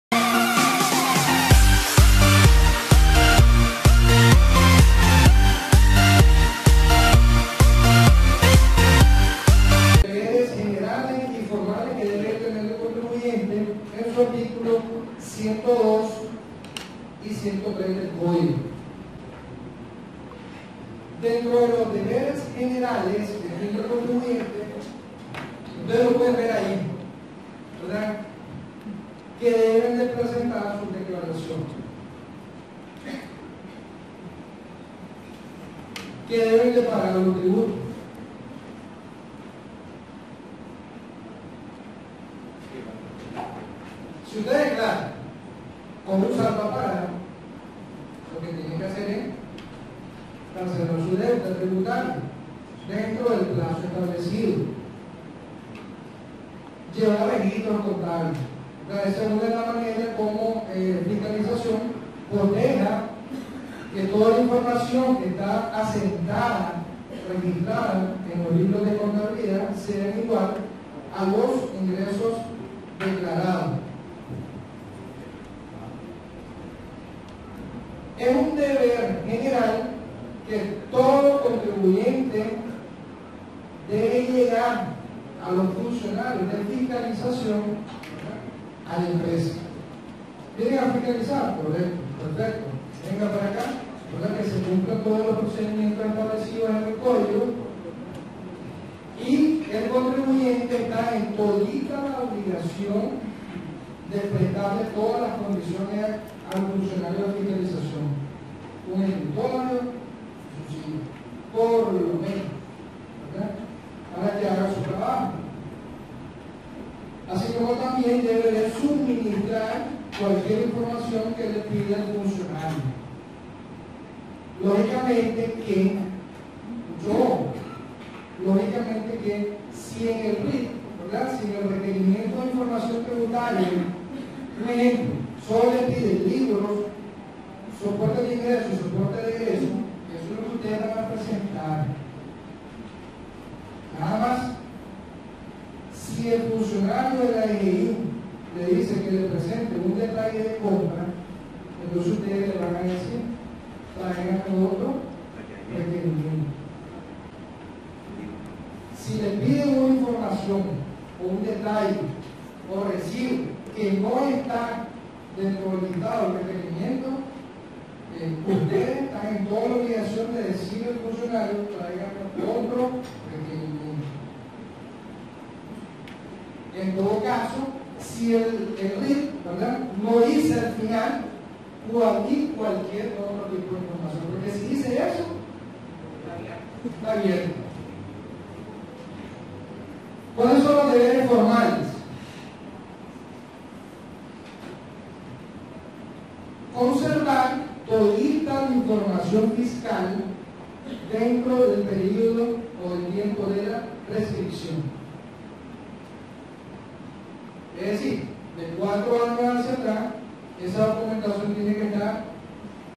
Deberes generales y formales de los Contribuyentes
Conferencia impartida en la Facultad de Ciencias Jurídicas y Sociales de la Unan León a estudiantes y profesionales del Derecho, economistas y Contadores Públicos y Financieros.

Con el único objetivo de dar a conocer a los participantes y a los suscriptores de mi canal cuales son los Deberes Generales y formales que tienen todos contribuyentes ante la Dirección General de Ingresos. Todo con base en los Art N°102 y 103 del CTr